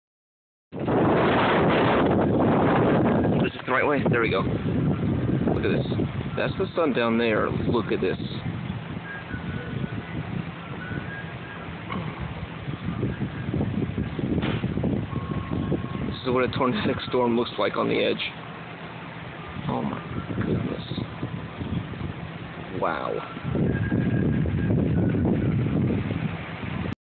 DFW Tornadic storm 5-24-2011
This was the trailing edge of a whole mess of supercells that blasted through DFW on May 24th 2011. What you are seeing is the light poking through the space between 2 of the Super Cells.